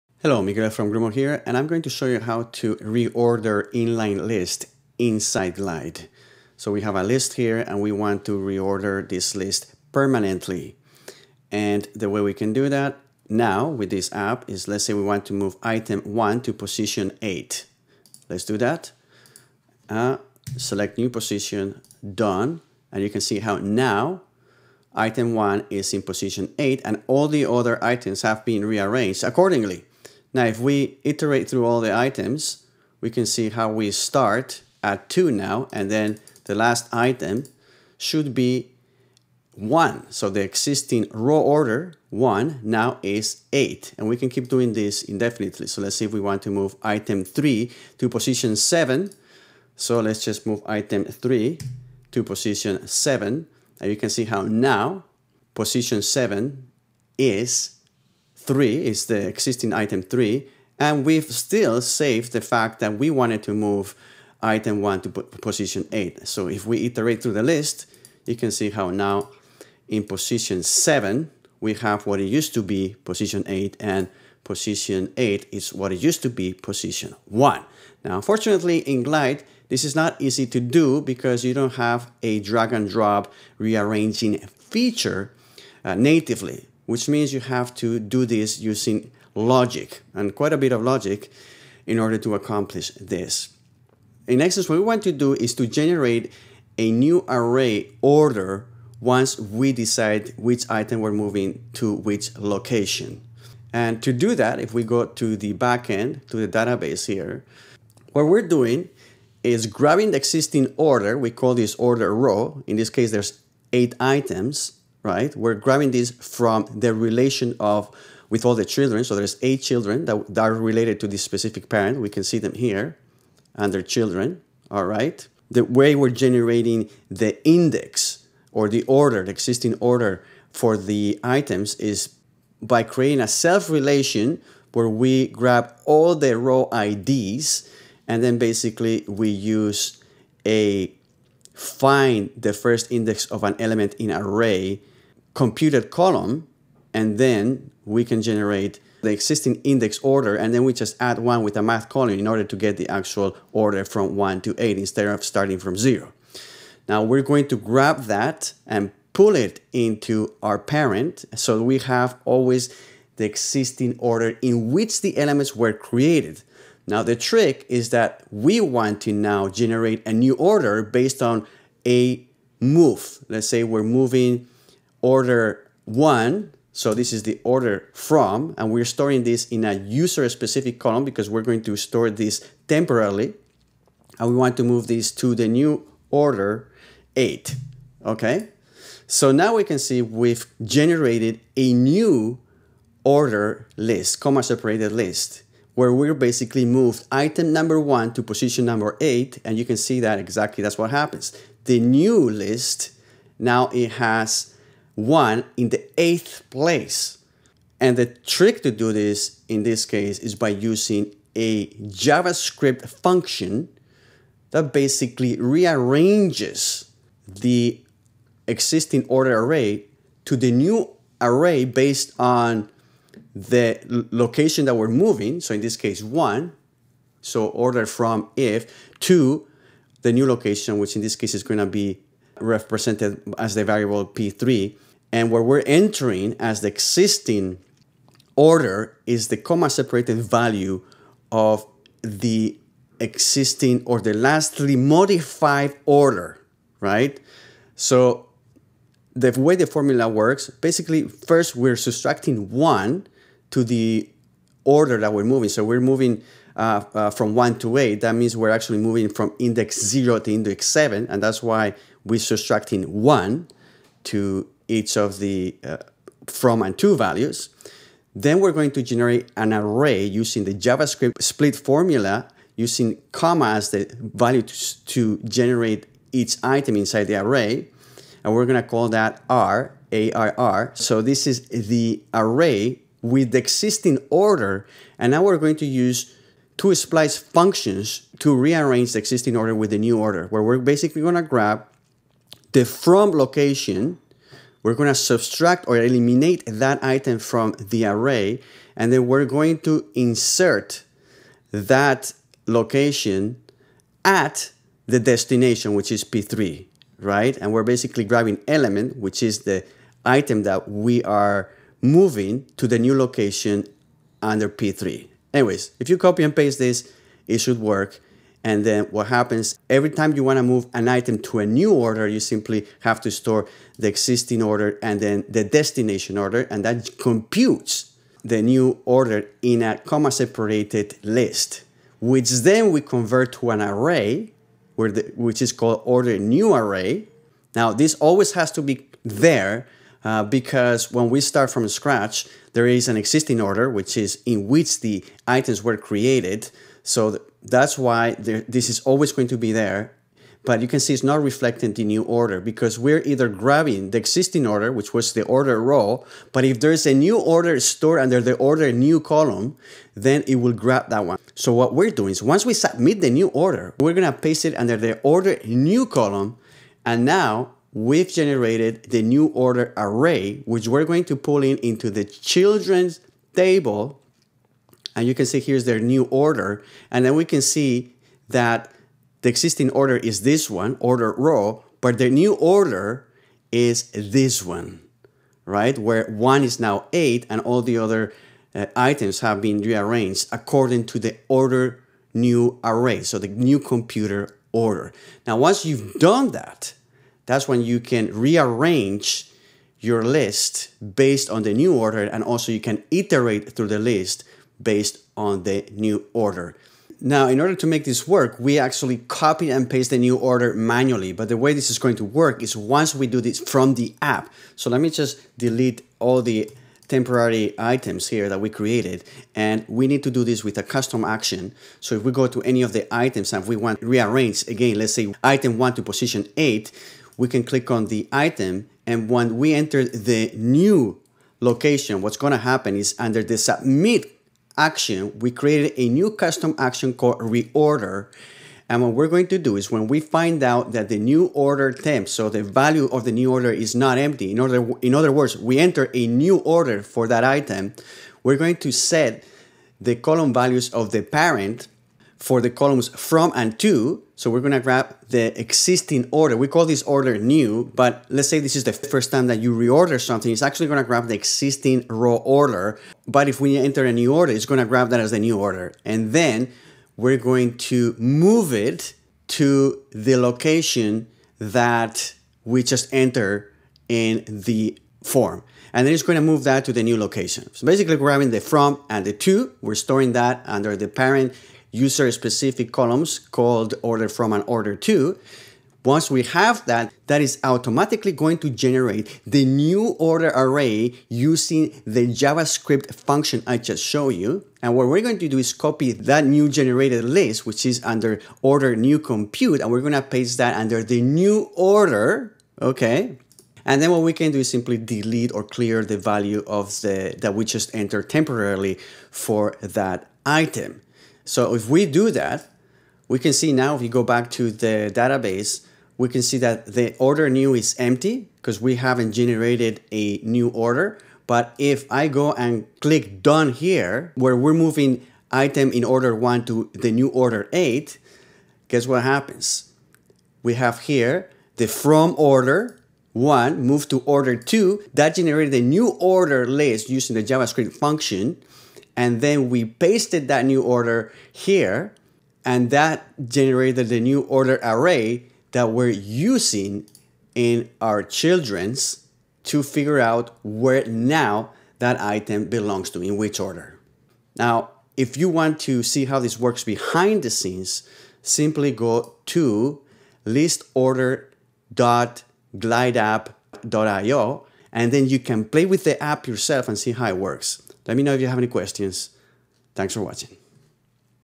List Reordering in Glide | Full Tutorial
Learn how to rearrange inline lists in Glide.

p2=p2-1;
p3=p3-1;

var arr = p1.split(",");

    var element = arr[p2];
    arr.splice(p2, 1);
    arr.splice(p3, 0, element);

var output = arr.join(",");

return output;

Miguel @GRUMO